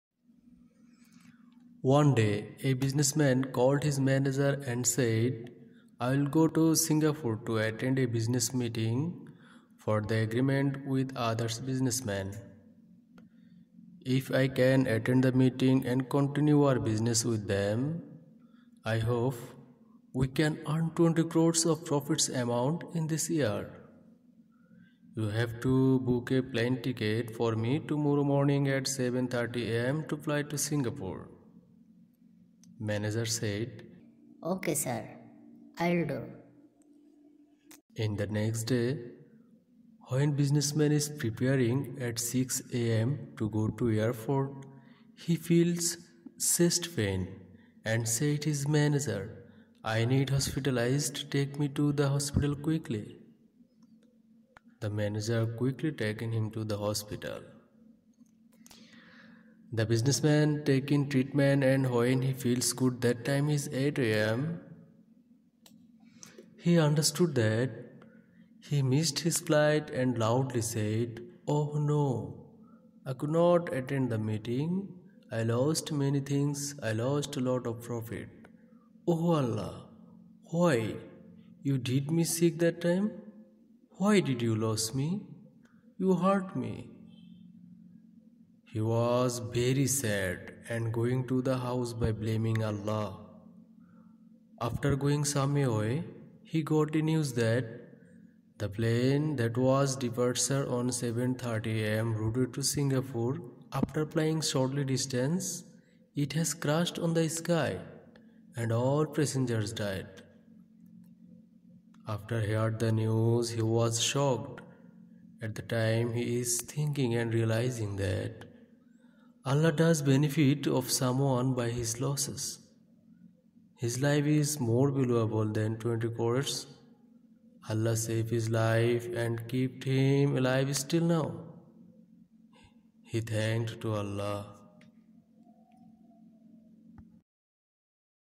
Allah Does Benefit of Someone by His Losses.
''When you lost something unfortunately, don't worry and don't get angry maybe you will get better thing than losing''.

One day a businessman called his manager and said: I'll go to Singapore to attend a business meeting for the agreement with others businessman. if I can attend the meeting and continue our business with them, I hope we can earn 20 crores of profits amount in this year.
You have to book a plane ticket for me tomorrow morning at 7:30 AM to fly to Singapore.

Manager said OKAY sir, I'll do.

In the next day, when businessman is preparing at 6:00 AM to go to airport he feels chest pain and said his manager “I need hospitalized take me to the hospital quickly.”

The Manager quickly taken him to the hospital.

the businessman taken treatment and when he feels good that time is 8:00 AM. He understood he missed his flight and loudly said: Oh no, I couldn't attend the meeting, I lost many things I lost lot of profit. oh Allah. why you did me sick that time? Why did you lose me? You hurt me.

He was very sad and going to the house by blaming Allah. after going some away, he got a news that, "The plane, that was departure on 7:30 AM routed to Singapore after flying shortly distance it has crashed on the sky and all passengers died."

After heard the news he was shocked.
At the time he is thinking & realizing that, Allah does benefit of someone by his losses.
His life is more valuable than 20 Crores. Allah safe his life and kept him alive still now.
He thanked to Allah…